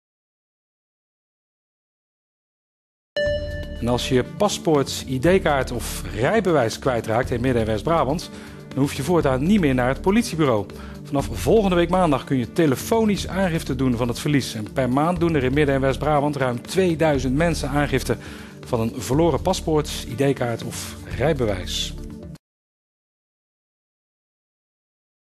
Telefonische aangifte vermist paspoort
Bij de politie in Midden- en West-Brabant kan vanaf volgende week telefonisch aangifte worden gedaan van een vermist of gestolen paspoort, identiteitskaart of rijbewijs. De politie stuurt het proces-verbaal digitaal naar de gemeente, zodat mensen niet naar het politiebureau hoeven.